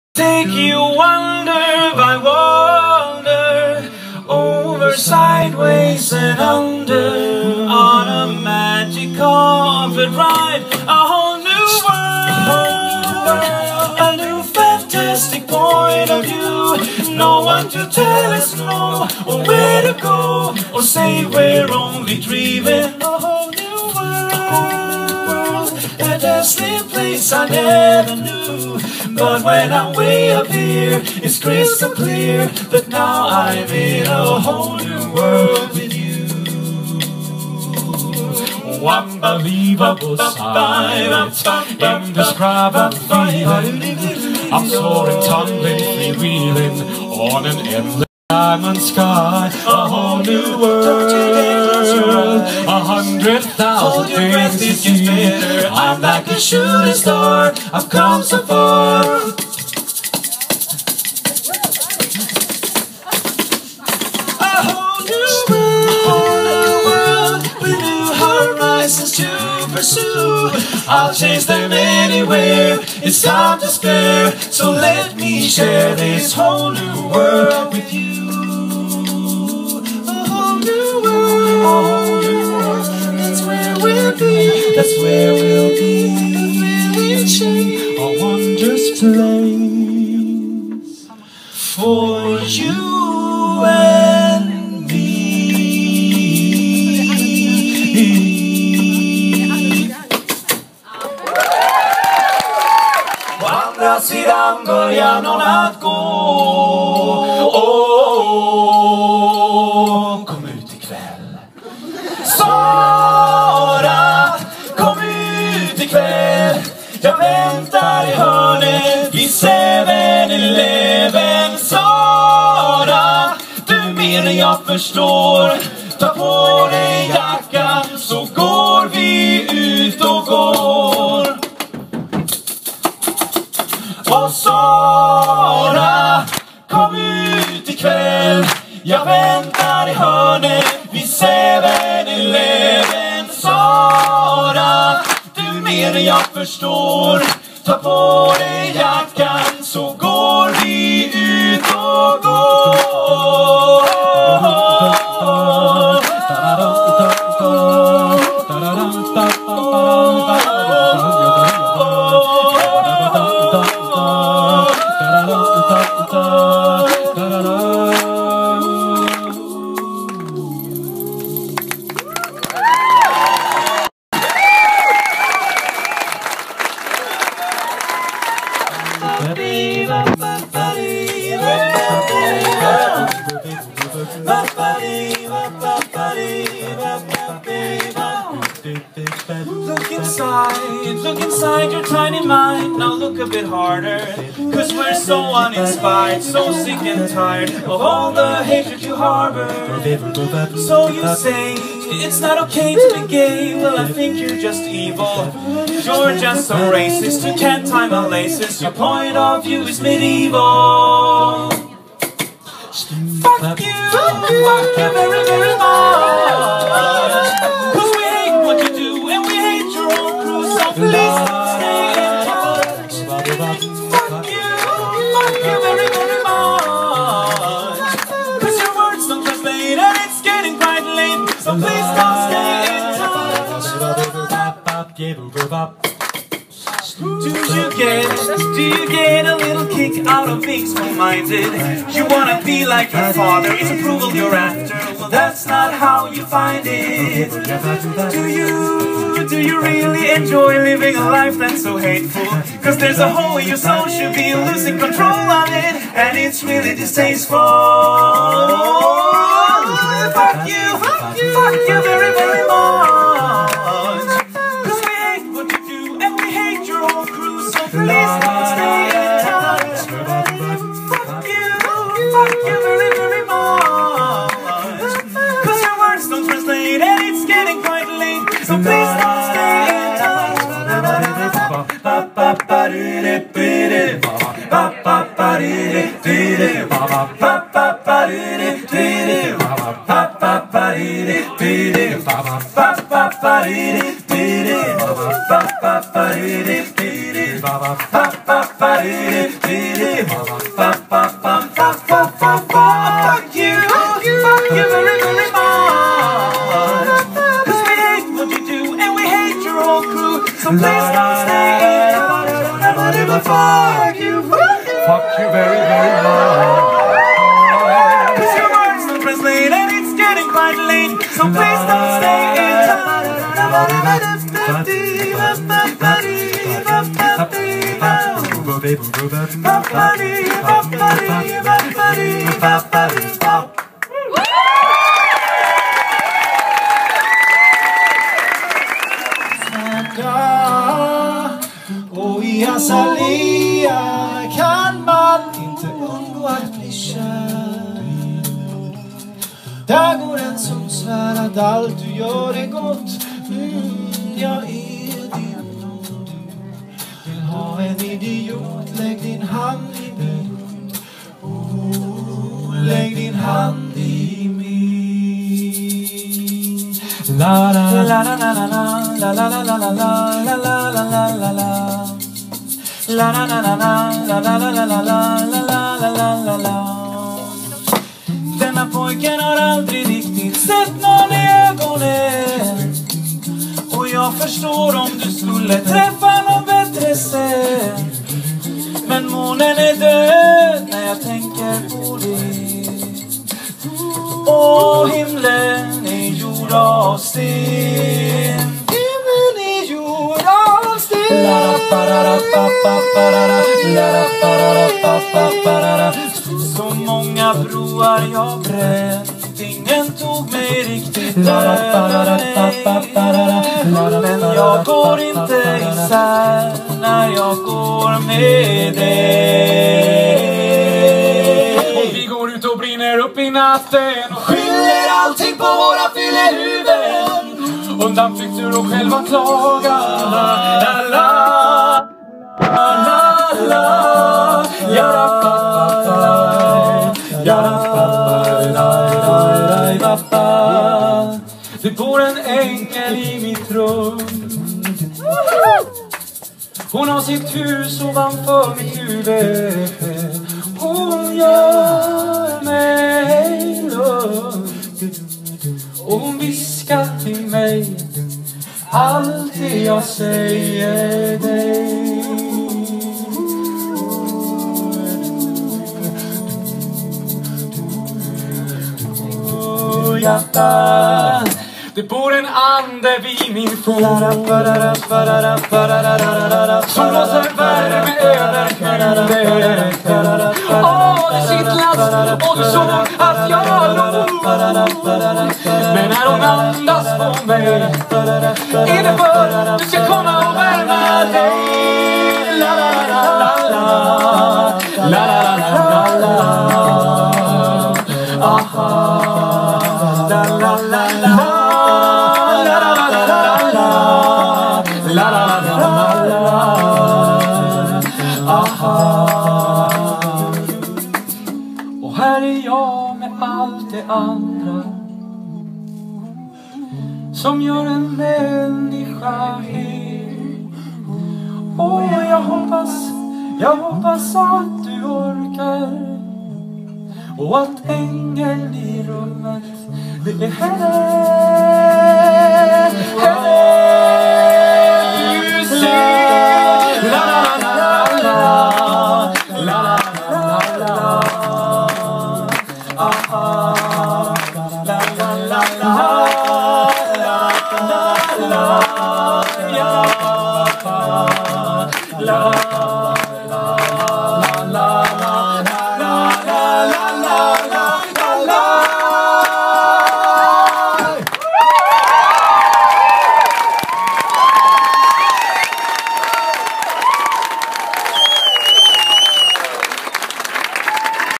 Solala @ EastWestSushi Örebro
Konsert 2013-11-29 på EastWestSushi, Örebro
Ett filmat medley av några melodier.